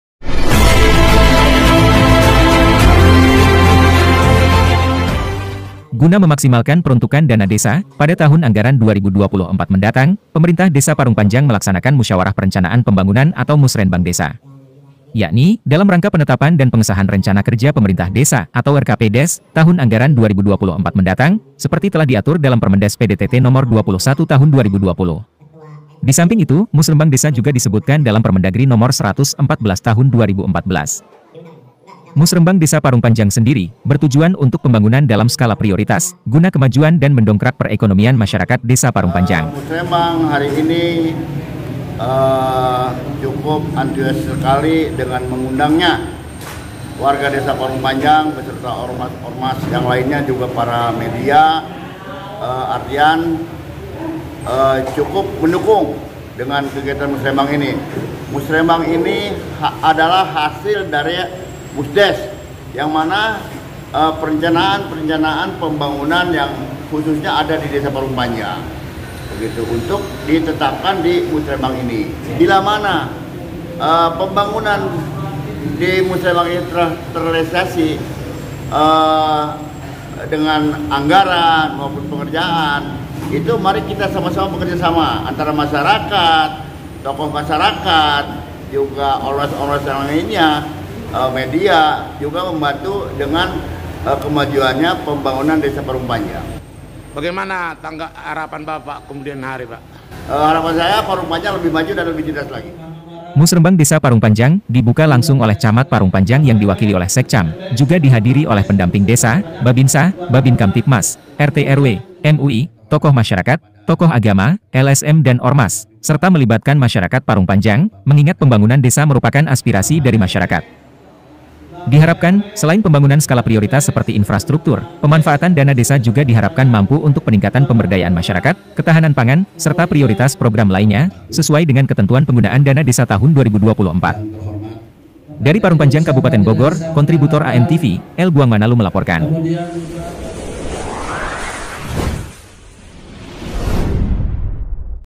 Antusiasnya Masyarakat Ikuti Musrembang Desa Parungpanjang di RKPDes 2024
Guna memaksimalkan peruntukkan dana desa pada tahun anggaran 2024 mendatang, Pemerintah Desa Parungpanjang  melaksanakan Musyawarah Perencanaan Pembangunan atau Musrenbang Desa. Yakni, dalam rangka penetapan dan pengesahan rencana kerja pemerintah desa atau RKP Des tahun anggaran 2024 mendatang, seperti telah diatur dalam permendes PDTT nomor 21 tahun 2020, disamping itu musrembang desa juga disebutkan dalam Permendagri nomor 114 tahun 2014.

Musrembang desa parungpanjang sendiri, bertujuan untuk pembangunan dalam skala prioritas, guna kemajuan dan mendongkrak perekonomian masyarakat desa parungpanjang

Musrembang Desa Parungpanjang,dibuka langsung oleh camat Parungpanjang yang diwakilioleh sekcam, juga dihadiri oleh Pendamping Desa, Babinsa, babinkamtibmas, RT RW, MUI, tokoh masyarakat, tokoh agama, LSM dan ormas,serta melibatkan masyarakat Parungpanjang, mengingat pembangunan desa merupakan aspirasi dari masyarakat.

Diharapkan, selain pembangunan skala prioritas seperti infrastruktur, pemanfaatan dana desa juga diharapkan mampu untuk peningkatan pemberdayaan masyarakat, ketahanan pangan,serta prioritas program lainya,sesuai dengan ketentuan penggunaan dana desa tahun 2024.